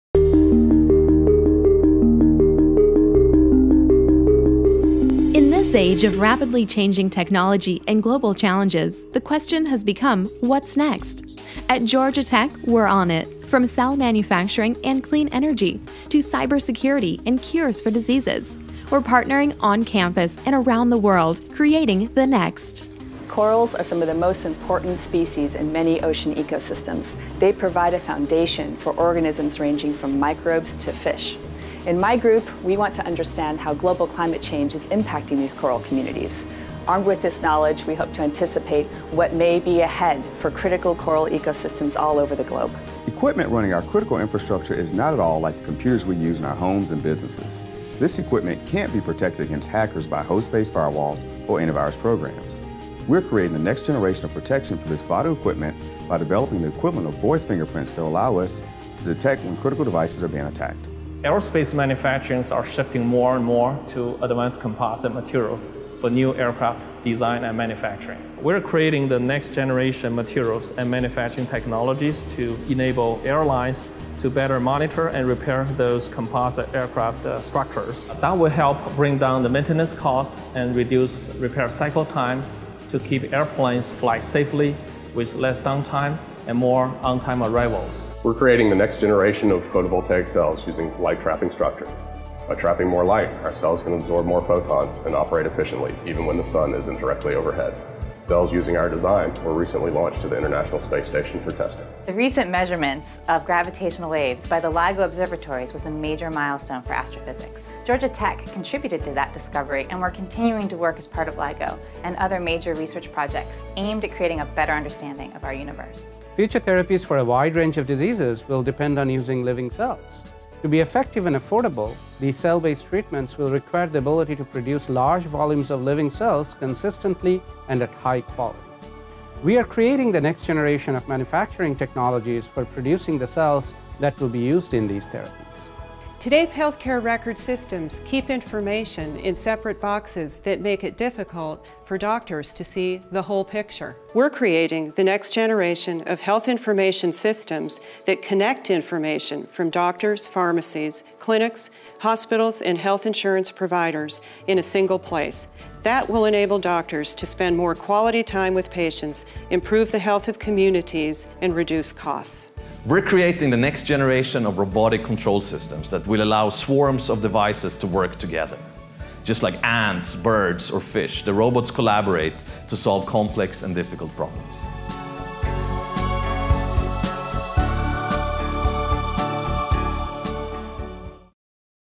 Cobb, Beyah, Zhang, Ready, Shoemaker, Roy, Wagner-Dahl and Egerstedt: Creating the Next Research
In this age of rapidly changing technology and global challenges, the question has become, “What’s next?” At Georgia Tech, we’re on it. From cell manufacturing and clean energy, to cybersecurity and cures for diseases, we’re partnering on campus and around the world to create solutions.

Experts Kim Cobb, Raheem Beyah,  Chuck Zhang, Jud Ready, Deirdre Shoemaker, Krishnendu Roy, Margaret Wagner-Dahl, and Magnus Egerstedt discuss exciting innovations impacting our future.